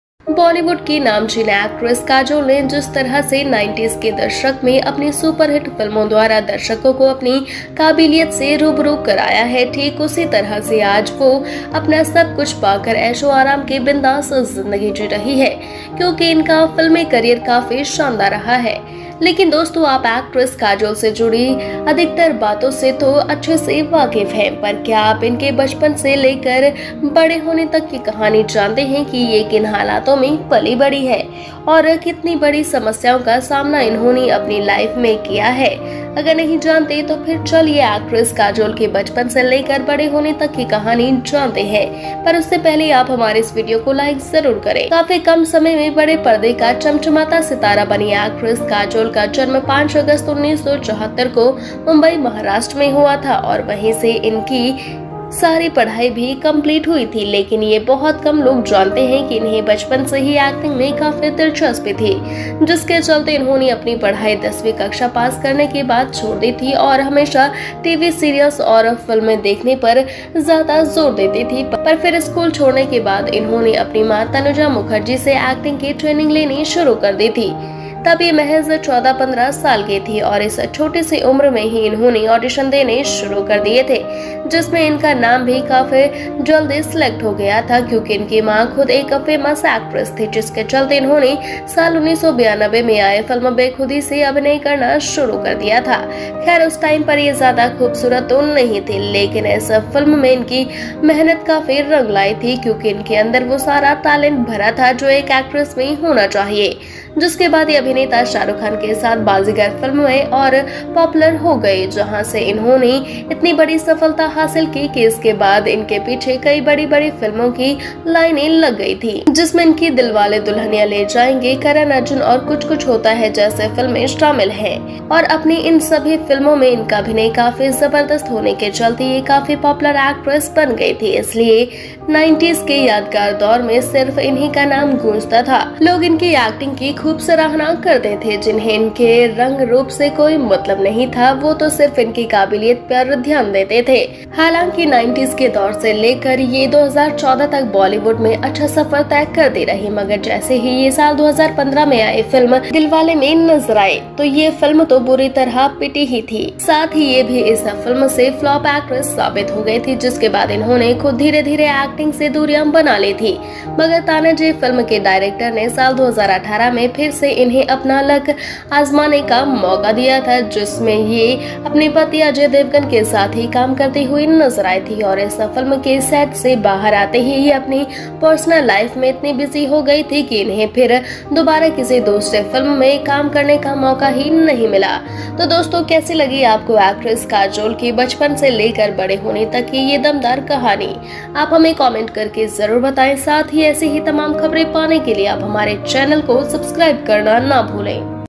बॉलीवुड की मशहूर अभिनेत्री काजोल का जानिए बचपन से लेकर बड़े होने तक का सफर..........
Bollywood_all_news_biography_Know_the journey_of_Bollywood's_famous_actress_Kajol from_childhood_to_growing_up.